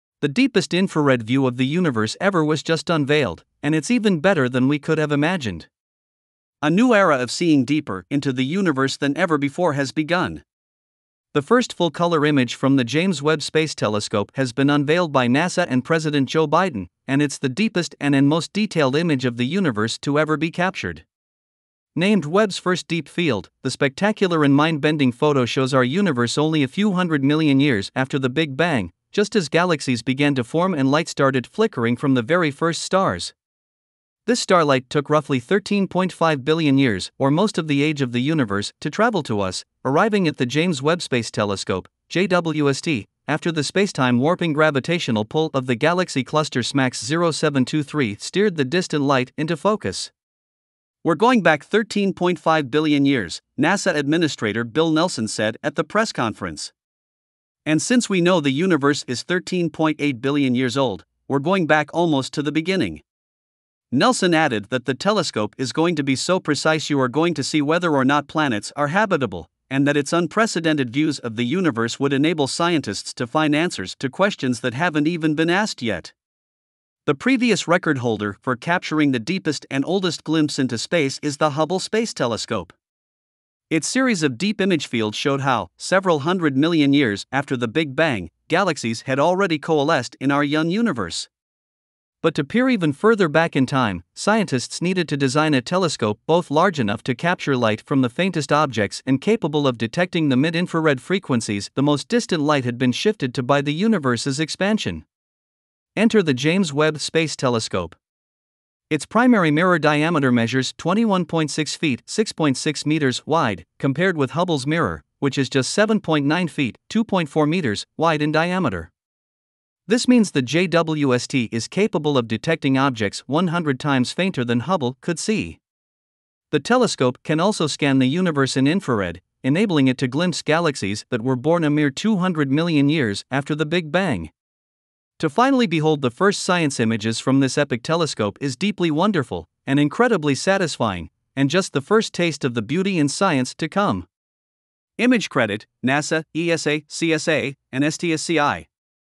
James Webb Space Telescope's 1st photos ultradeep view of the universe
Credits:
Image credit: NASA, ESA, CSA, and STScI

 james webb james webb space telescope jwst update james webb space telescope images jwst new photos